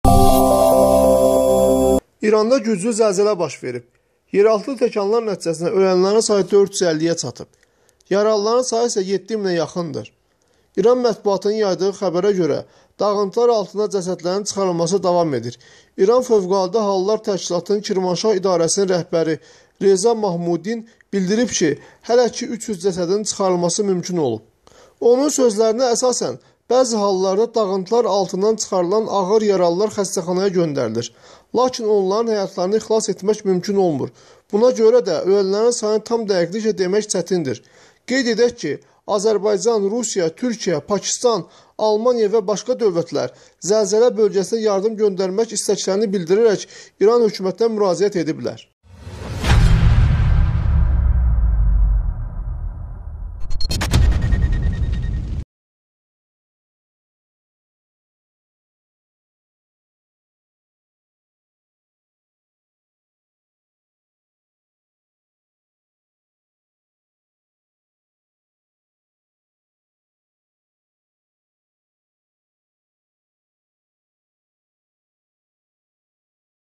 İranda güclü zəlzələ - 450 ölü: ölənlərdən 300 nəfərin cəsədinin çıxarılması mümkün olub
İranda güclü zəlzələ baş verib. Yeraltı təkanlar  nəticəsində ölənlərin sayı 450–yə çatıb, yaralıların sayı isə 7 minə yaxındır.  İran mətbuatının yaydığı xəbərə görə  dağıntılar altından cəsədlərin çıxarılması davam edir.
İranın Fövqəladə Hallar Təşkilatının Kirmaşah idarəsinin rəhbəri Reza Mahmudian bildirib ki, hələlik 300 cəsədin çıxarılması mümkün olub. Onun sözlərinə əsasən, bəzi hallarda dağıntılar altından çıxarılan ağır yaralılar xəstəxanaya göndərilir, lakin onların həyatlarını xilas etmək mümkün olmur. Buna görə də ölənlərin sayını tam dəqiqliklə demək çətindir. Qeyd edək ki, Azərbaycan, Rusiya, Türkiyə, Pakistan, Almaniya və başqa dövlətlər zəlzələ bölgəsinə yardım göndərmək istəklərini bildirərək İran hökumətinə müraciət ediblər.